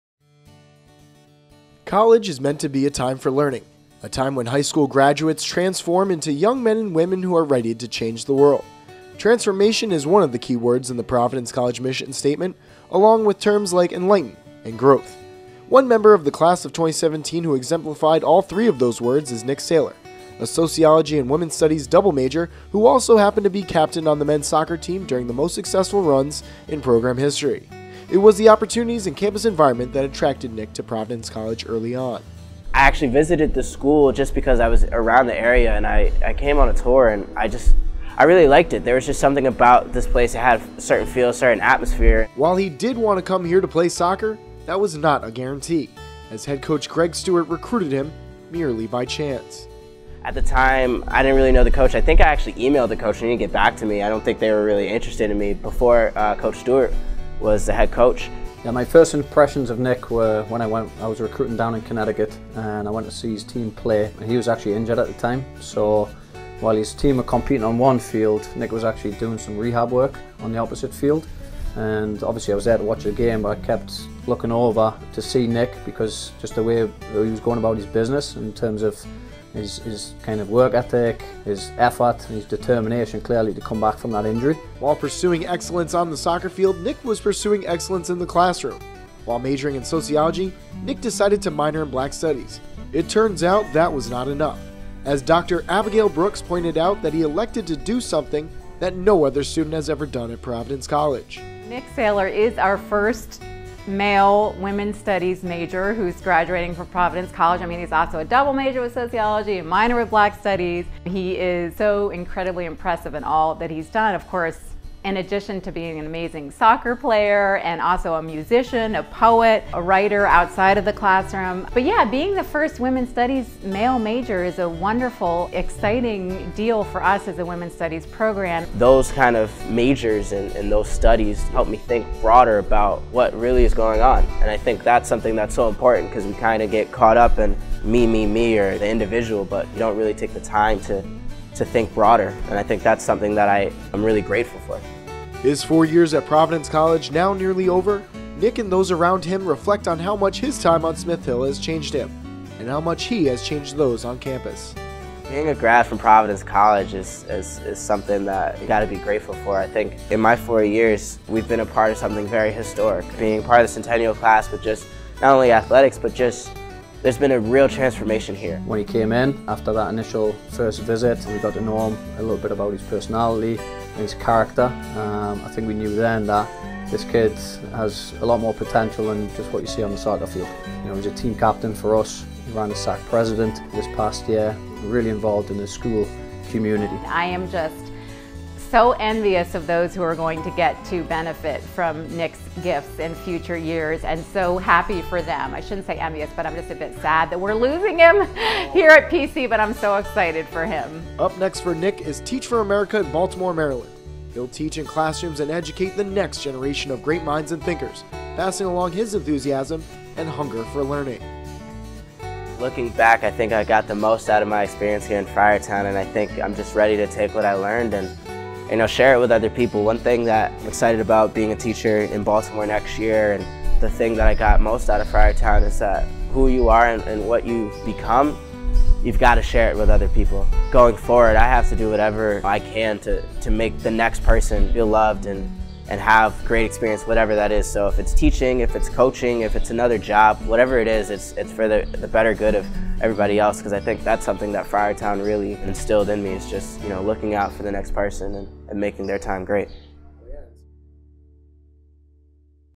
Nick Sailor: More Than A Soccer Player
Nick Sailor ’17 (Bloomfield, Conn.) passed along a lot more than a soccer ball during his undergraduate career at Providence College. A four-year member of the men’s soccer team and a co-captain his senior year, he distinguished himself on the field and off, including the classroom. He double-majored in sociology and women’s studies, carried a minor in black studies, and will graduate as the first male women’s studies major in College history. Sailor can reflect on a legacy of exceptional accomplishment and character as his days as a Friar undergraduate dwindle. Known for his upbeat manner and work ethic, he served as president of the Student-Athlete Advisory Council and as a Friar Foundations mentor, sang with the Footprints Gospel Choir, and earned two awards as a student-athlete this academic year: the Department of Athletics’ Mal Brown Award for sportsmanship, courage, and honor, and the soccer team’s Father Moseley Champion of Character Award. Sailor now will pass along his talents, knowledge, and spirit to schoolchildren in Baltimore, where he has committed to work with Teach For America.